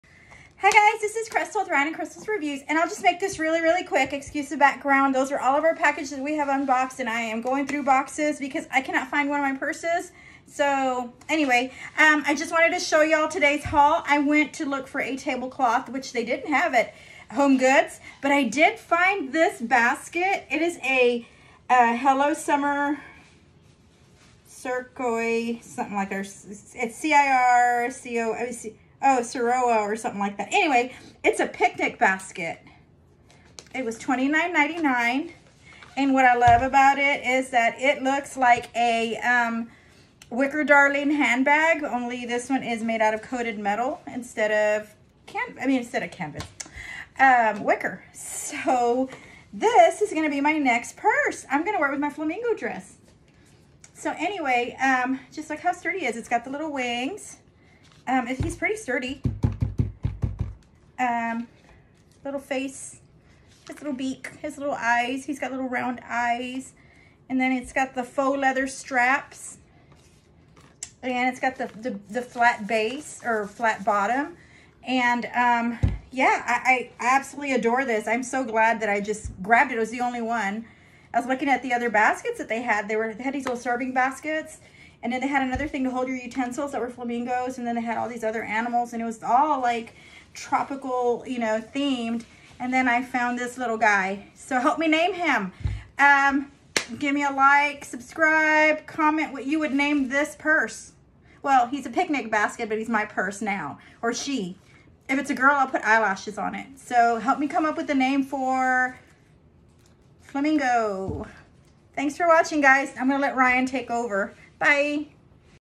Wow!! Flamingo Wicker Picnic Basket find from Home Goods!
Omg! Look at this amazing picnic basket! It’s a flamingo made of coated wire. I will use this as a purse! Looks similar to a Wicker Darking bag for 1/10th it the cost! Best $30 spent!

Found this at Home Goods in the picnic/housewares section!

Help me name the “bag”! Drop a suggestion in comments!

Have a super day!

Outfit:
Dress - ( Toy Story ) Stitch Shoppe
Eyeglasses - Kate Spade
Chicken necklace - ShinyCquin